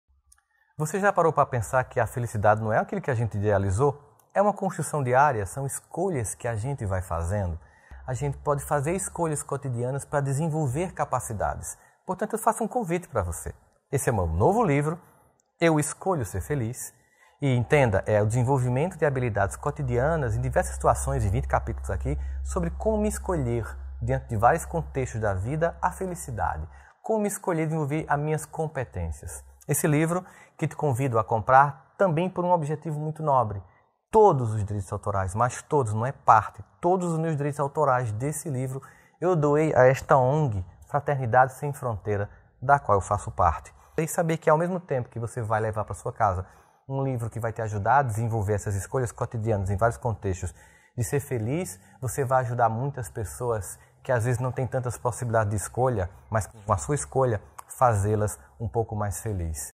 Eu Escolho Ser Feliz - Rossandro Klinjey
ROSSANDRO KLINJEY, psicólogo clínico, fenômeno nas mídias sociais e consultor do Programa Encontro com Fátima Bernardes, nos dá uma dica de como escolher a felicidade.